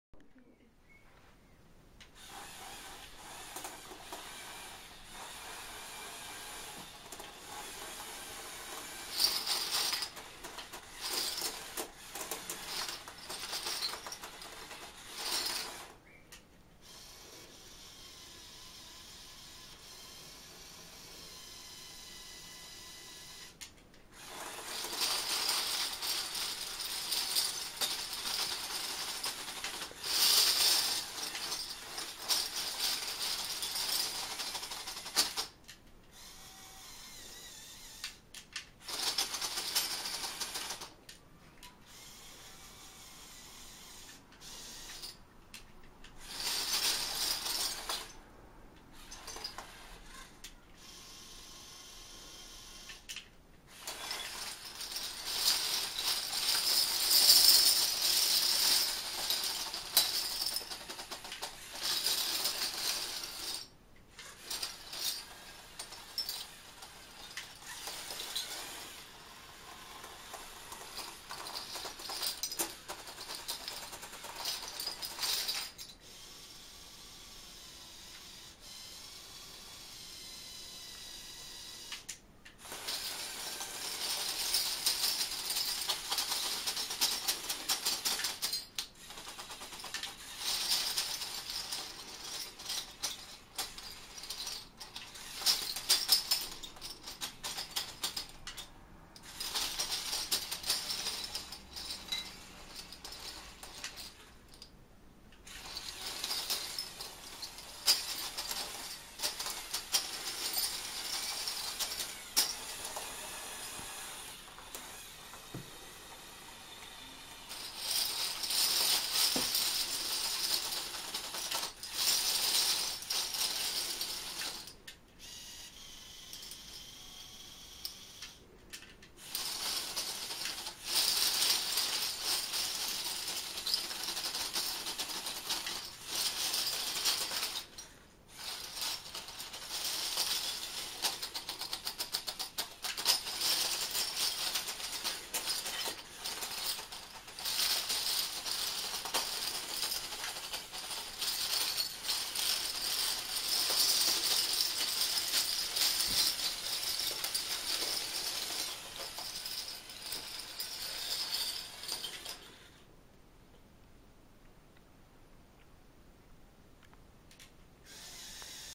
Biggest bottle cap (kroonkurken) pyramid ever! -You tube record-
This is my first bottle cap video ever. I made a pyramid, just for fun. It took 7 hours to build and its 20x20. There are 2870 botlle caps in. It's a you tube record because the biggest bottle cap pyramid on you tube was 19x19 . I let my lego technic bulldozer "topple"it. I'm glad to hear what you think.